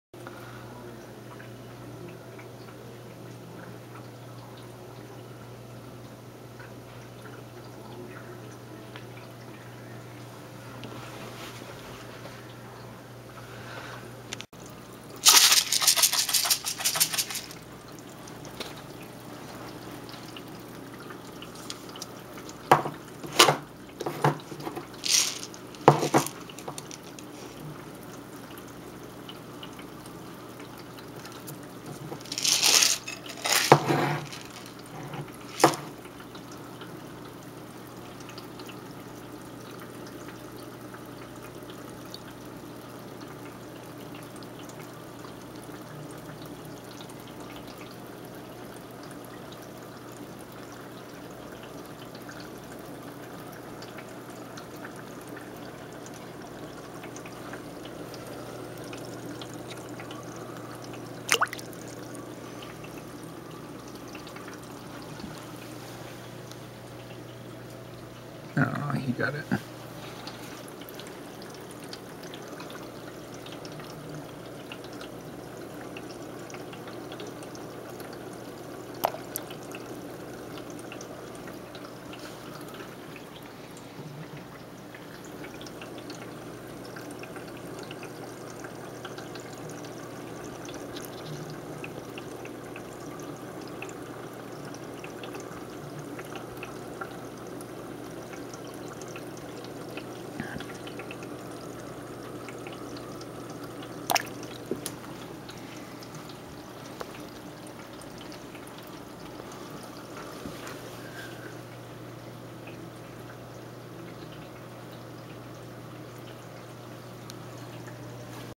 Hand feeding aquarium fish
Tinfoil barbs and a random. giving them food